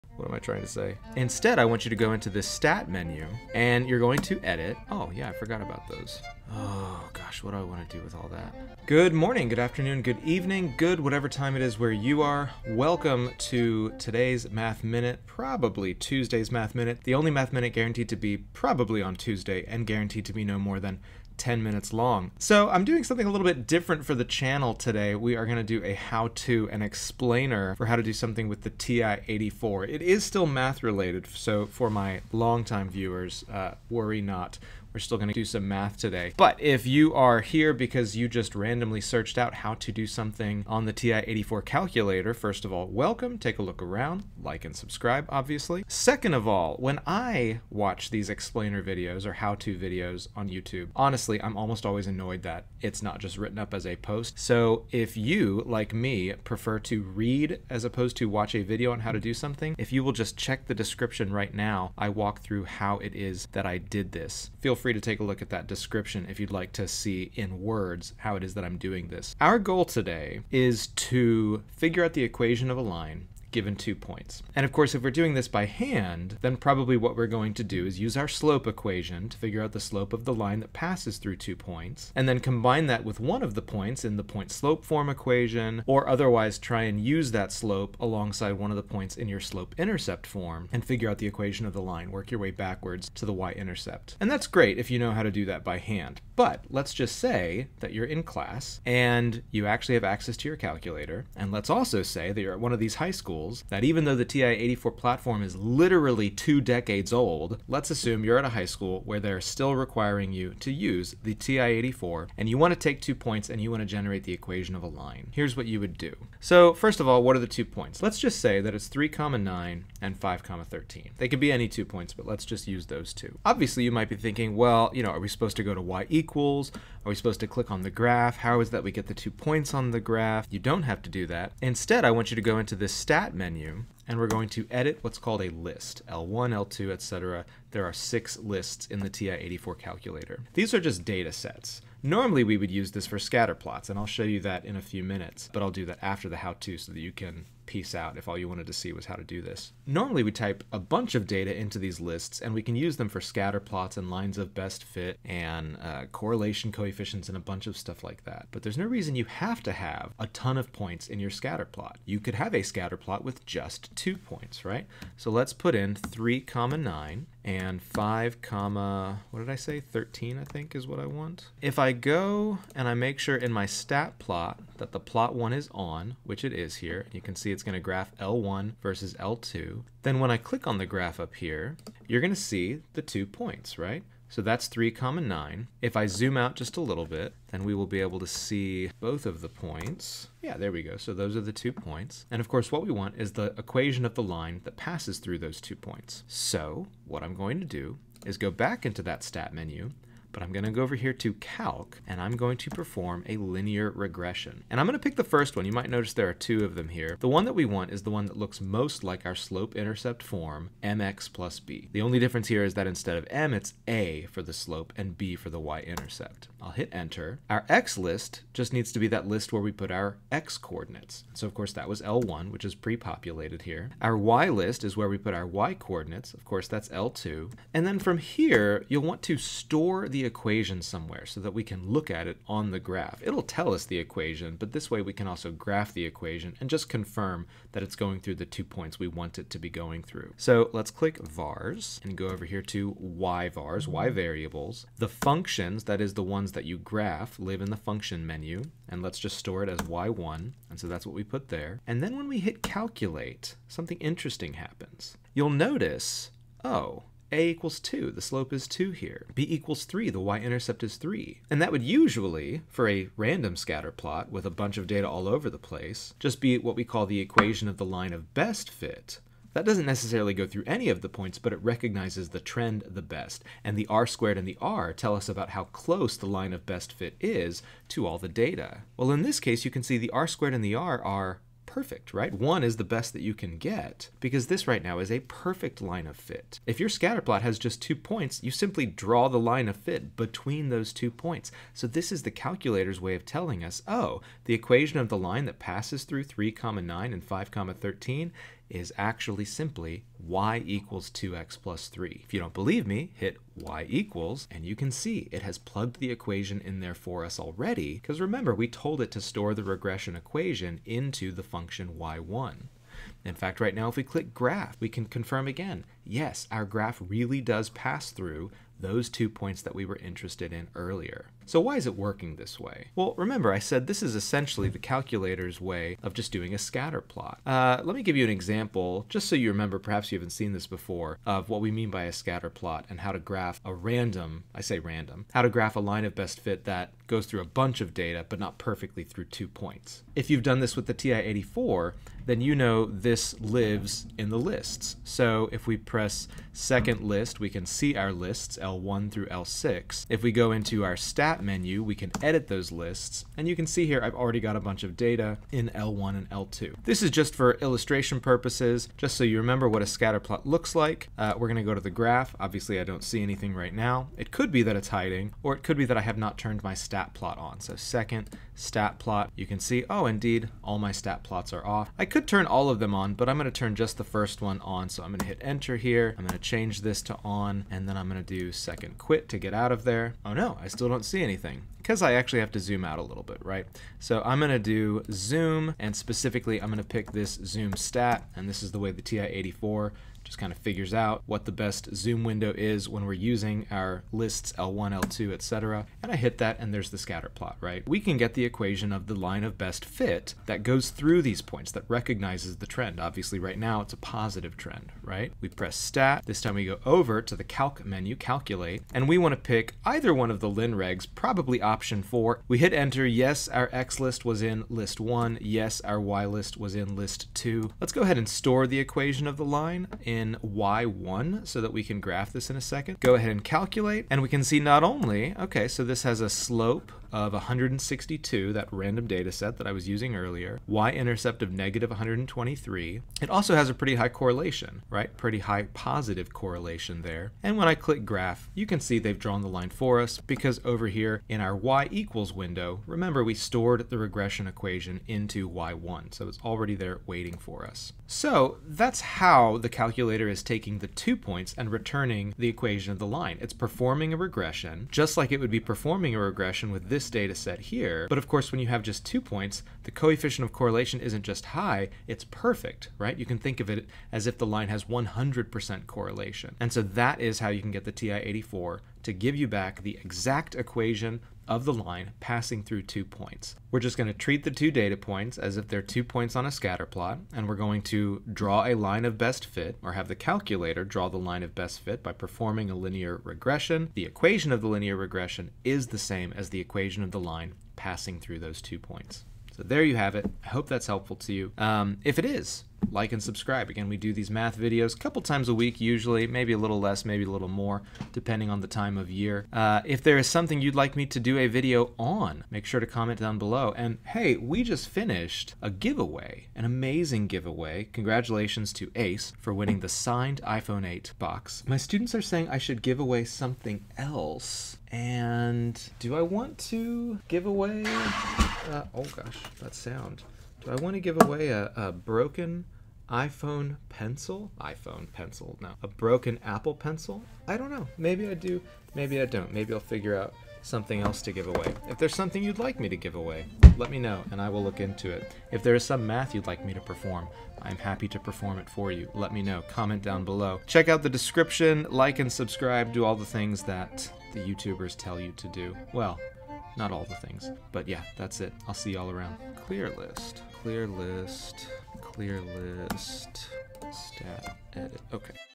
Compute the Slope-Intercept Equation of a Line on the TI-84 Calculator // Math Minute [#25][ALGEBRA]
As promised in the video, I’m going to do a bit of a longer description here to outline the process for taking two points as an input and having the TI-84 calculator produce the equation of the line for you. This method will work anywhere you can perform a linear regression on two points and have it give you the equation for the line of best fit (so, the various TI-Nspire models, Desmos.com, etc.), though of course the various commands will be different.

The basic idea is exactly what I outlined above: we’re going to take the fact that the calculator will perform a linear regression on a data set and use that to produce a “line of best fit” for a data set with just two points. Since that line will actually have perfect fit (any two points describe exactly one line), its equation will be the same as the equation of the line passing through those two points.

The first step in the TI-84 is to go into the “stat” menu. This is the button just to the left of the D-pad. From this menu, you can both edit the lists where we will enter in the ordered pairs and perform the linear regression itself. First, we’ll stay on the “EDIT” screen and selection option 1. Edit…

From there, you should have what looks like a spreadsheet with six columns: L₁, L₂, L₃, and so on. We want to use those first two columns like a table for our x- and y-coordinates. In the video, I use (3,9) and (5,13) as the examples, but of course you can use whatever two points you need the line to pass through. Type the x-coordinates into L₁ and the y-coordinates into L₂.

Now, press the “stat” button again to access the calculations sub-menu. This is just to the right of “EDIT” and says “CALC”. You can get there simply by pressing right. In this submenu, you can see a variety of statistical calculations. What we want is option 4. LinReg (ax+b). You’ll notice there is another LinReg that looks slightly different (a+bx). It makes no difference which you use, but the ax+b is most like the typical mx+b slope-intercept form.

From the LinReg screen, make sure your Xlist points to L₁ and your Ylist to L₂. If you typed the data into different columns, of course, you’ll need to change that. If you want to store the equation of the line itself or access its graph later, you can place Y₁ into the Store RegEQ spot. You can access Y₁ from the “vars” button (in the “Y-VARS” submenu).

Finally, calculate, and you’re there. Your TI-84 has calculated the equation of the line. Congratulations!